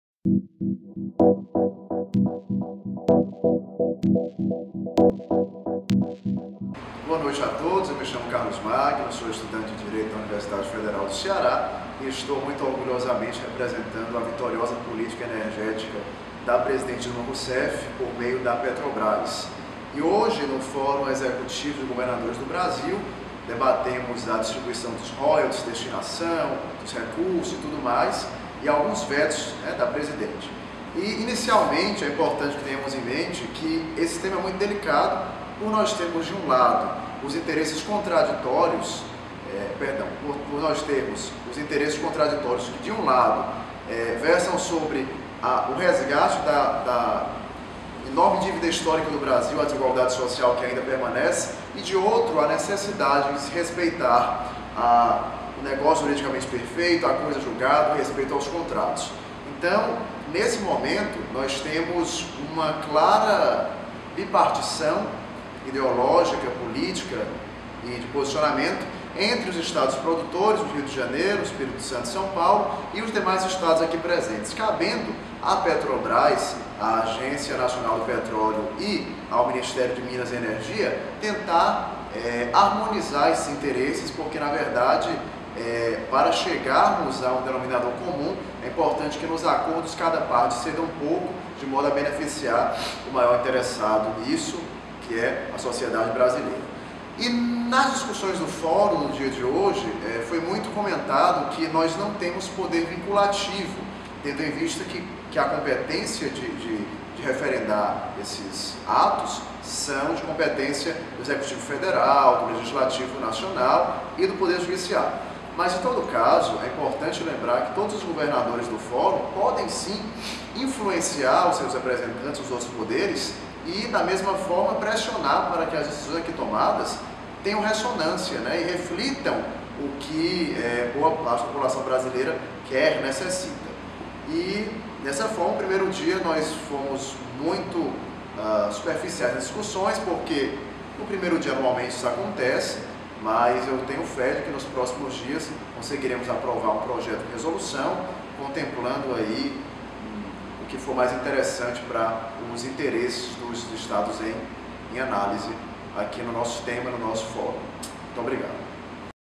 UniSim 2013 - 1º dia - FEGB - Carlos Magno
As primeiras impressões e um resumo do primeiro dia de debates no FEGB pela ótica do estudante de direito Carlos Magno.